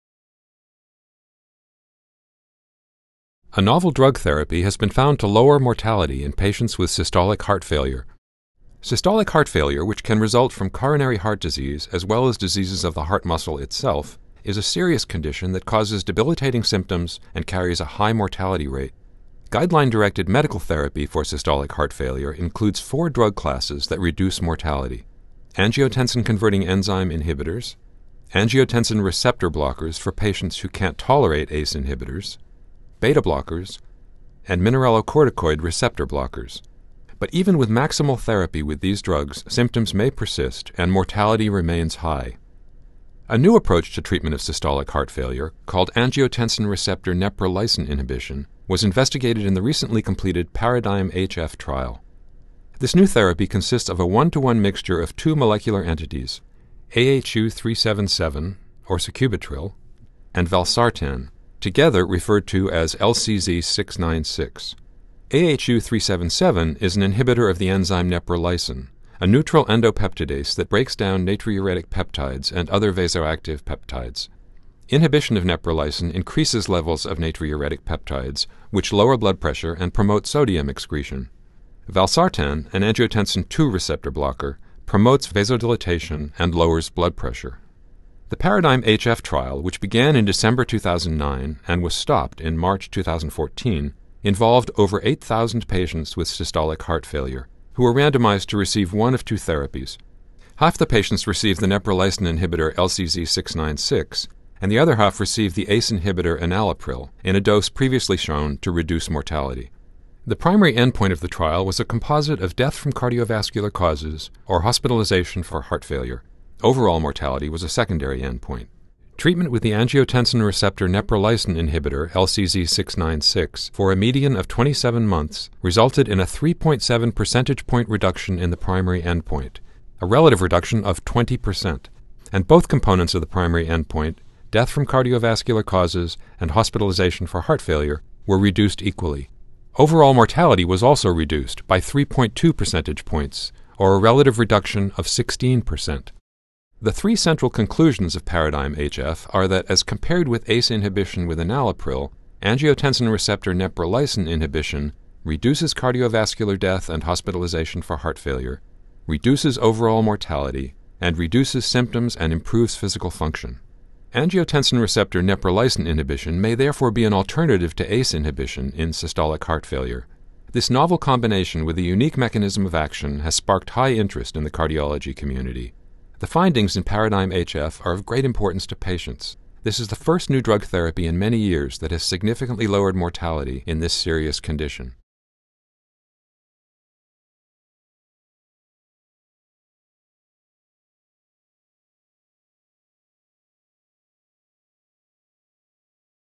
PARADIGM-HF: A Concise Summary with Journal Club Questions
A three-minute summary of the PARADIGM-HF trial includes five Journal Club questions.

See the NEJM article, "Angiotensin–Neprilysin Inhibition versus Enalapril in Heart Failure"

See the related editorial, "Neprilysin Inhibition — A Novel Therapy for Heart Failure"

See the related Perspective article: "Paradigm Shifts in Heart-Failure Therapy — A Timeline"